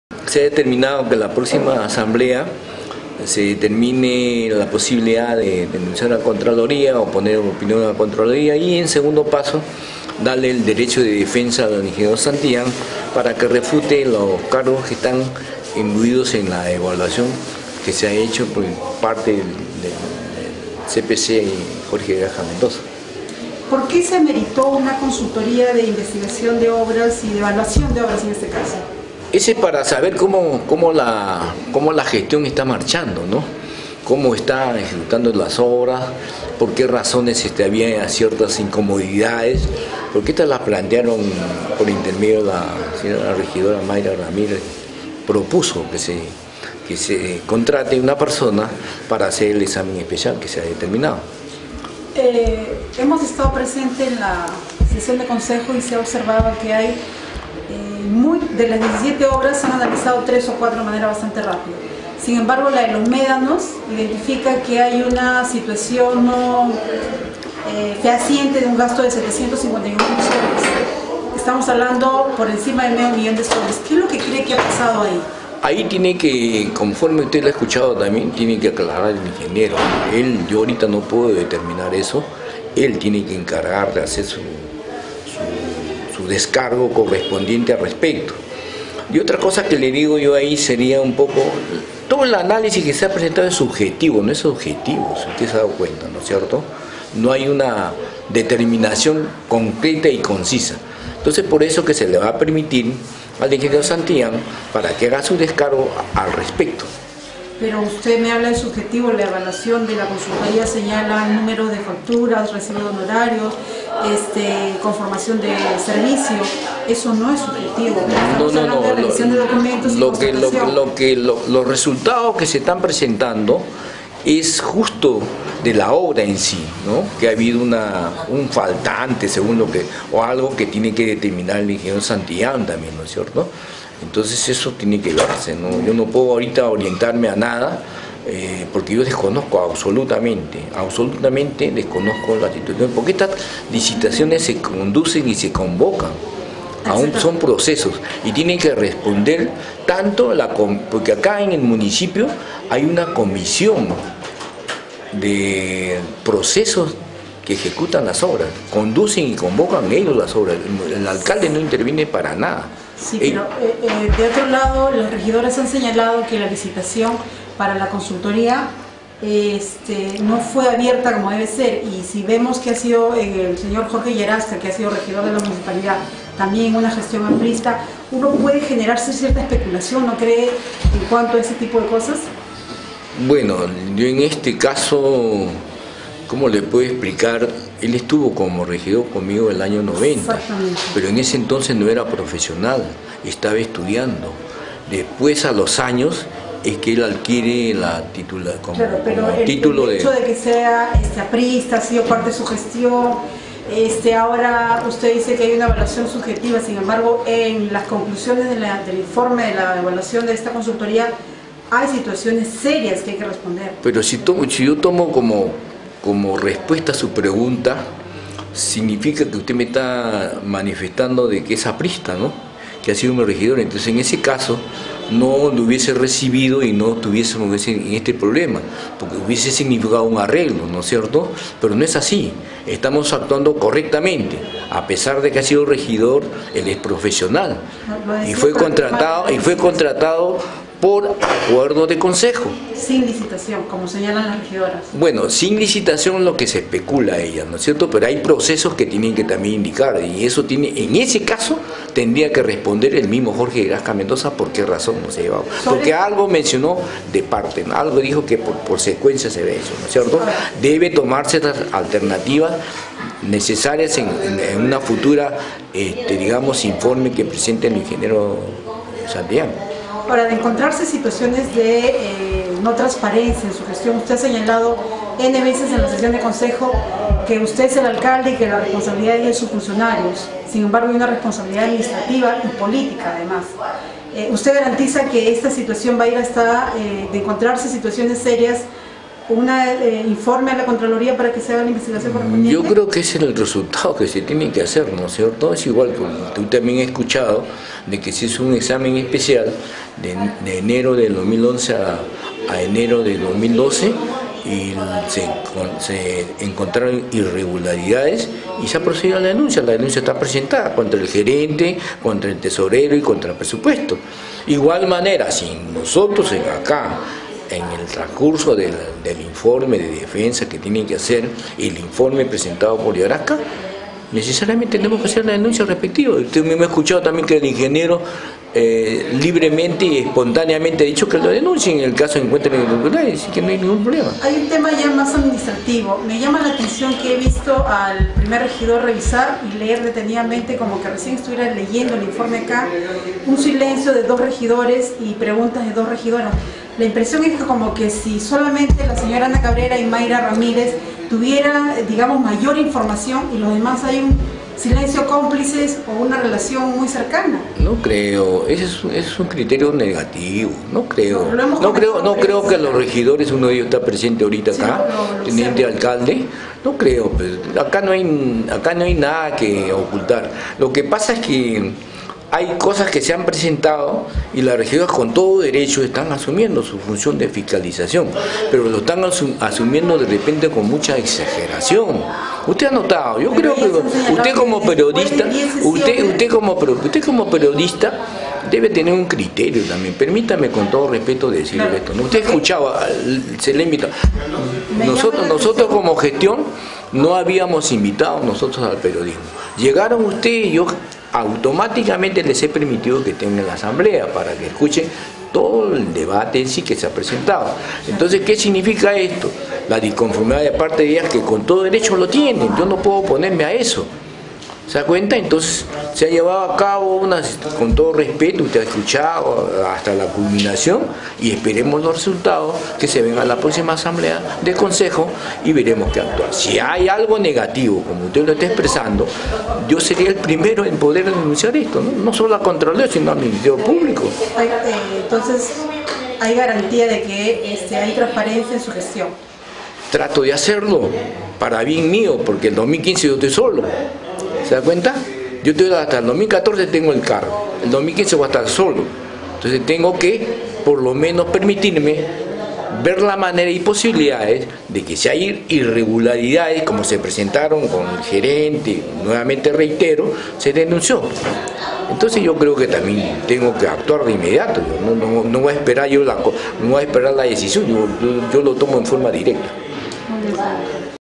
Alcalde Subtanjalla responde sobre obras con desbalances financieros
Pecho manifiesta que el encargado de responder sobre este tema es el ingeniero Daniel Santillán Li, jefe de obras y director del departamento de desarrollo urbano de la municipalidad cuando se ejecutaron estas obras.
Finalmente el burgomaestre manifestó que de confirmarse regularidades tomará la decisión de elevar este tema a la Contraloría de la República para su intervención. Sin embargo; será en sesión de consejo del día viernes 05 a las 10:00 am en donde se decidirá si este informe presentado pasa a manos de dicho organismo contralor.